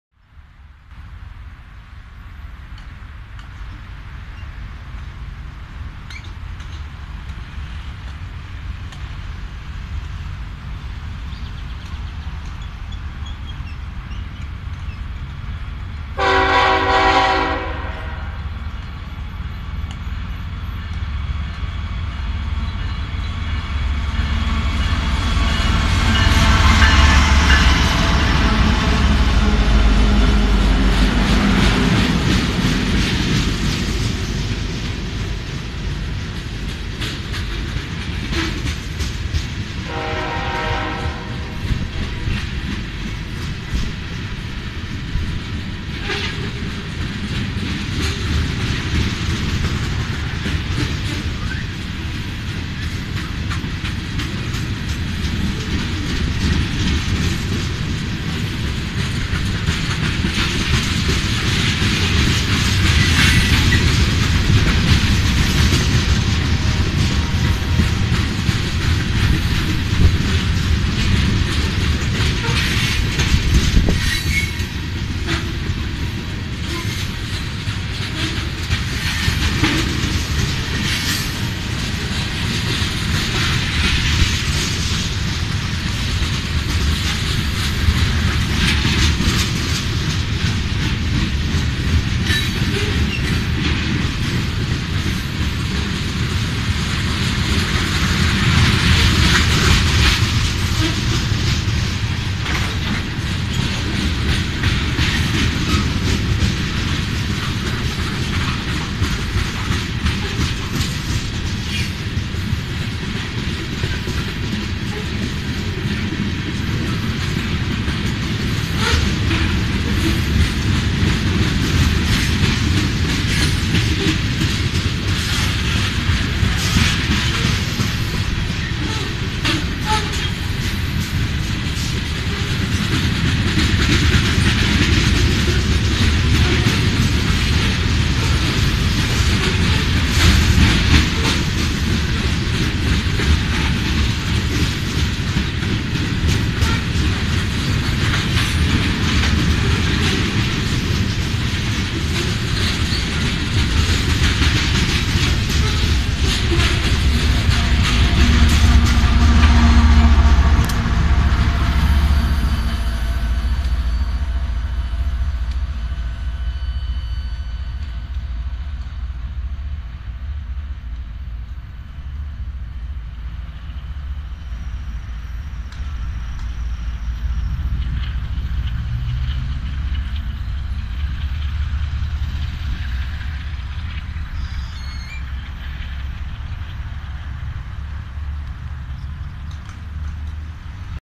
Tanquero Ferromex Nº 4812, 4005 & DPU End 4669 con 81 Carros (14/12/2016) Aguascalientes/Ags
Tren de Carga con 3 Locomotoras Ferromex N° 4812, 4005 & DPU End 4669 con 81 Carros.
Tipo de Vagones: Cisterna.
Día: Jueves 15 de Diciembre 2016.
Ubicación: Avenida Manuel Gómez Morín a un Costado del Parque de las Tres Centurias en Aguascalientes Aguascalientes, México.